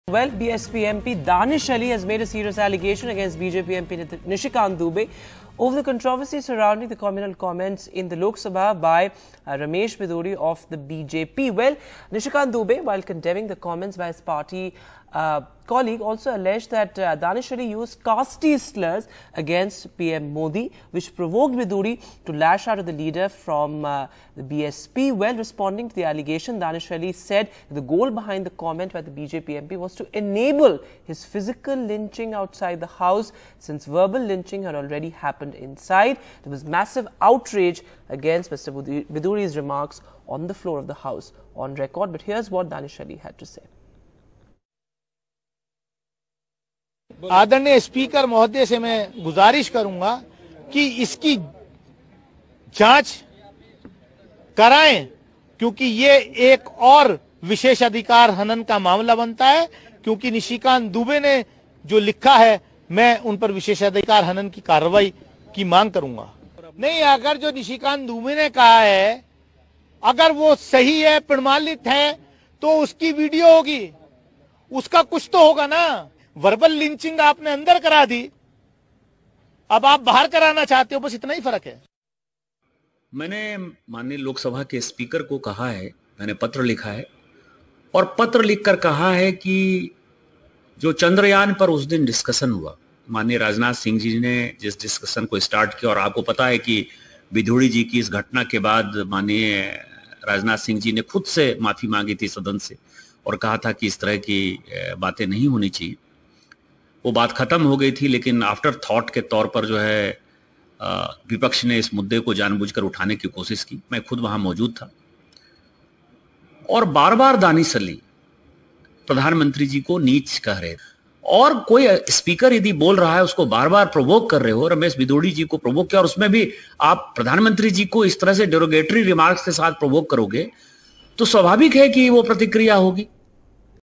MP Danish Ali On BJP Leader's Letter: "Creating Narrative For My Lynching"
Bahujan Samaj Party MP Danish Ali has made a serious allegation against BJP MP Nishikant Dubey over the controversy surrounding the communal comments in the Lok Sabha by Ramesh Bidhuri, another BJP MP.